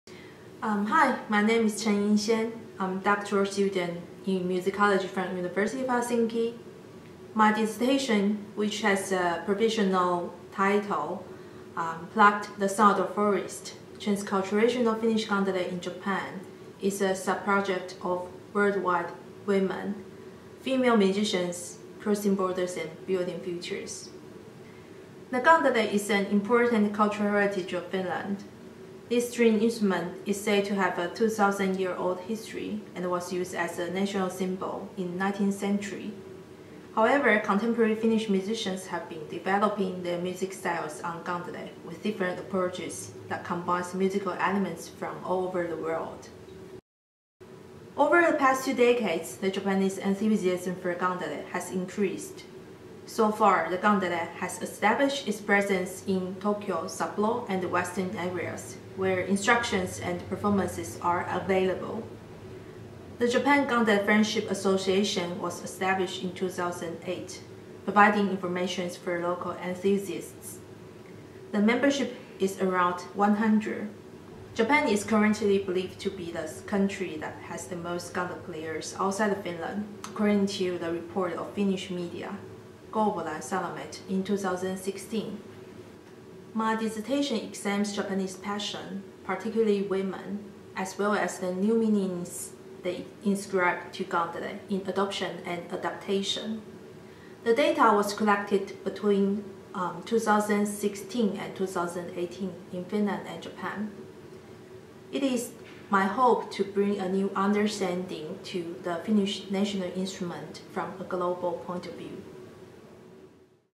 Subproject introduction: Transculturation of Finnish kantele in Japan
Music researcher Chen Ying Hsien talkes about her doctoral dissertation “Plucking the sound of the forest”: Transculturation of Finnish kantele in Japan, which is a subproject of Woman World Wide – Female Musicians Crossing Borders and Building Futures.

Filmed and edited by Emmi Salervo